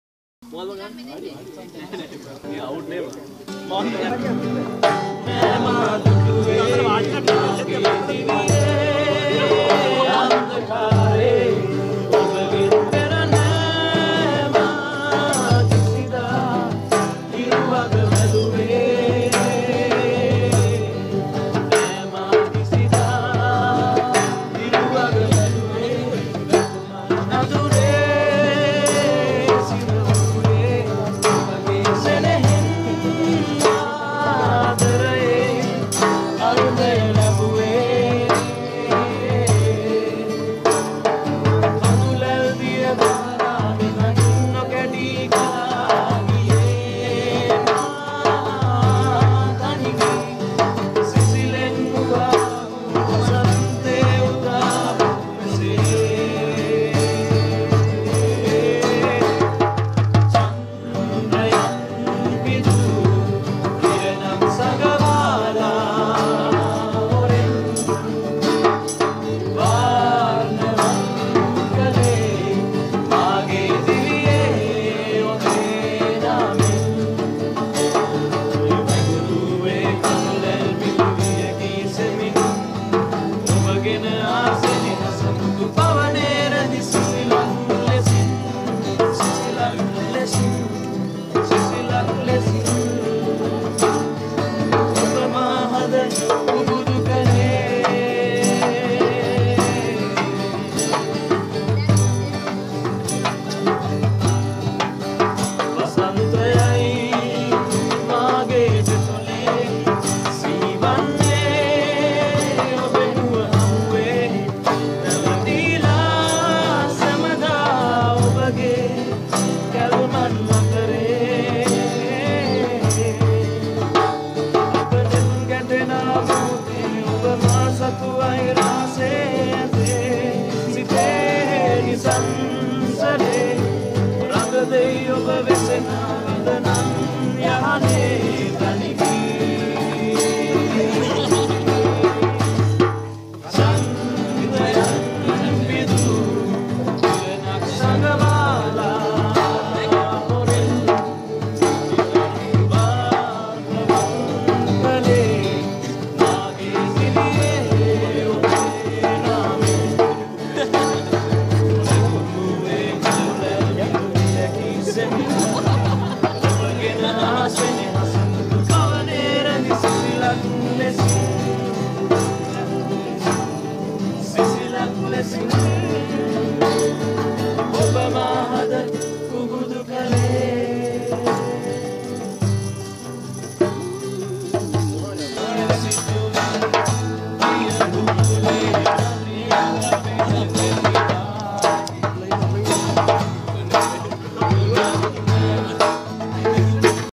2002 Grade 5 Batch of Gothami Junior Shool
this is get together party which was grade 5 gothami school batch.  at gampaha  botanical garden.
This is my Friends and Leisure